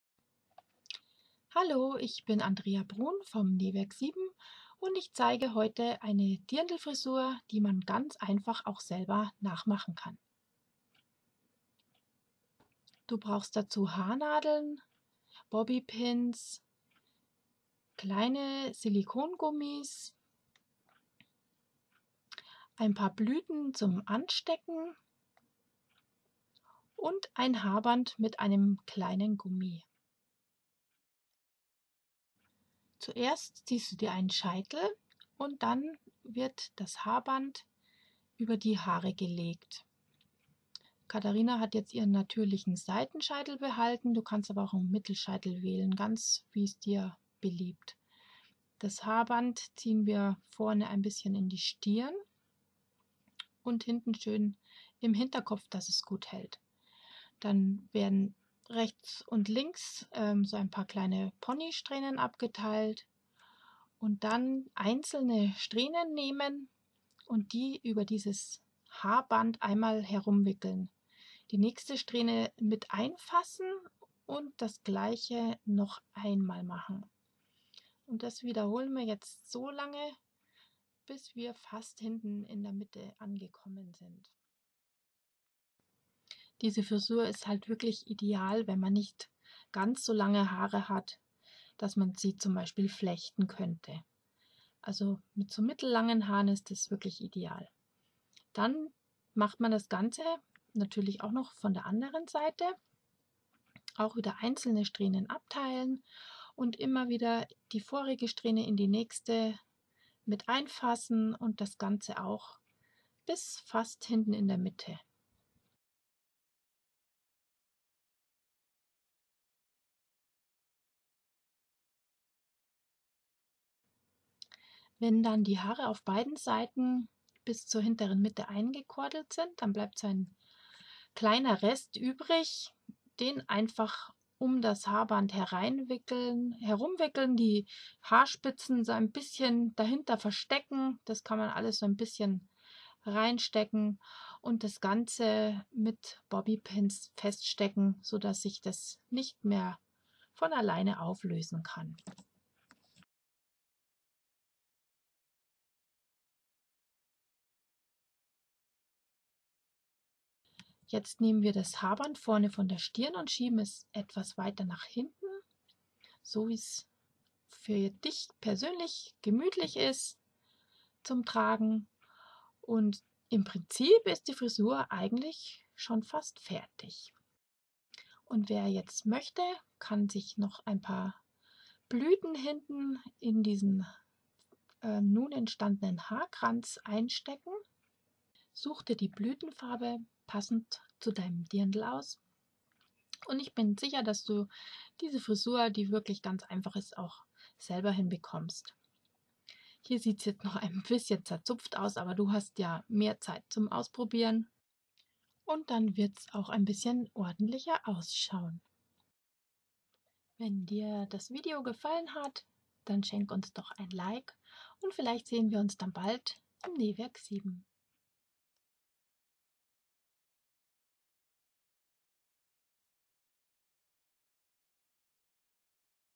Frisur zum Dirndl für kurze/halblange Haare- Nähwerk7
Eine ganz einfache Frisur zum Dirndl! Ideales Hairstyling für's Oktoberfest!